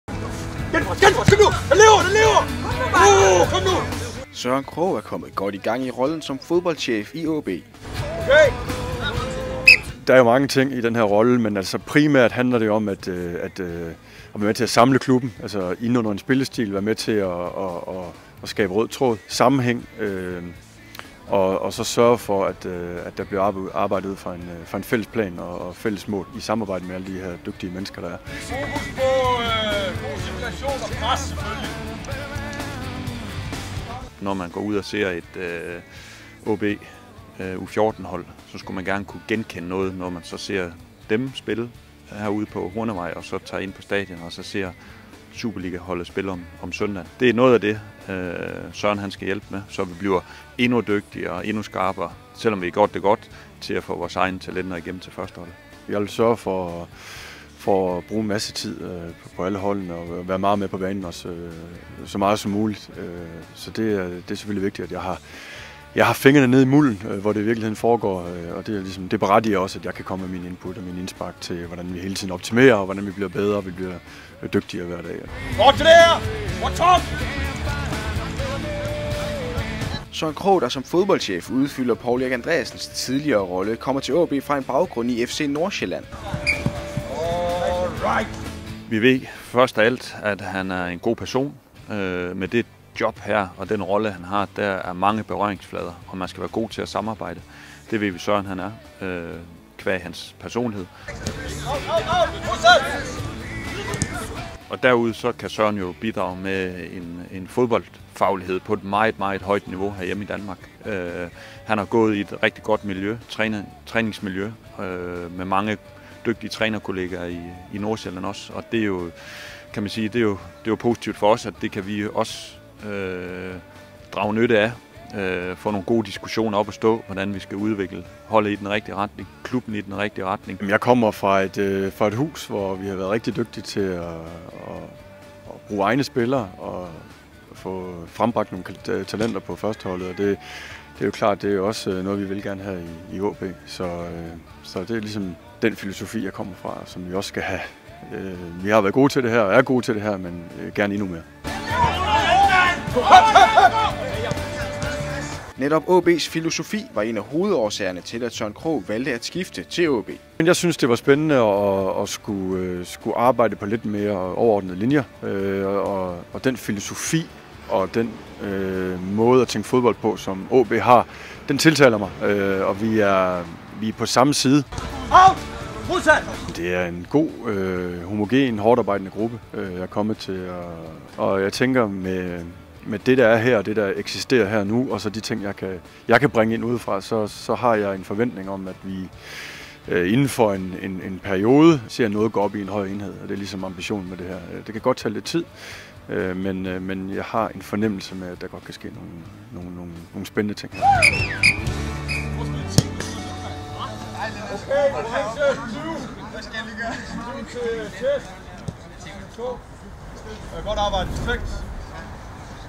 Søren Krogh - AaB's fodboldchef
Ved årets begyndelse tiltrådte Søren Krogh som fodboldchef i AaB.

AaB TV har talt med Søren Krogh, der kommer til Hele Nordjyllands Hold efter flere år i FC Nordsjælland. I dette indslag kan du høre hans tanker om AaB, stillingen som fodboldchef og den opgave der ligger foran.

Her skal han sammen med de nuværende trænere i Superligatruppen og talentafdelingen blandt andet skabe en sammenhængskraft fra ungdomsholdene til Superligatruppen, så AaB bliver endnu bedre til at få egne talenter til at slå igennem på førsteholdet.